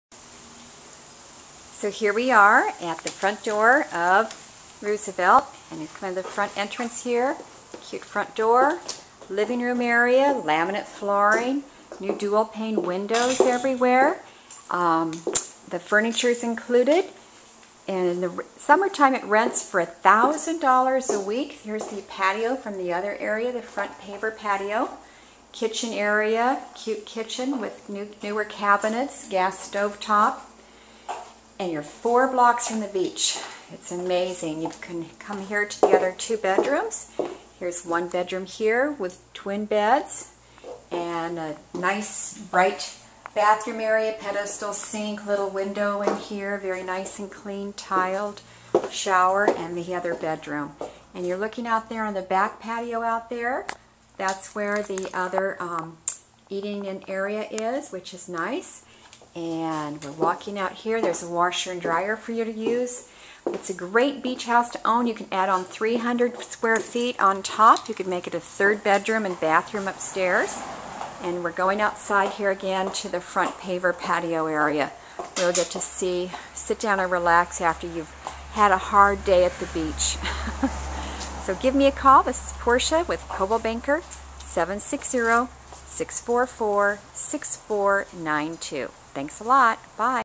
Roosevelt St, Carlsbad. Cute beach cottage with tons of potential!
This is a great opportunity. to have your own beach casita. Check out the zoning---residential or commercial. Want a great rental?? This cutie is currently booked for short term weekly rentals for summer!  Only 5 blocks to beach. Walk to Pizza Port and Paon Restaurant, downtown Carlsbad shopping, and more..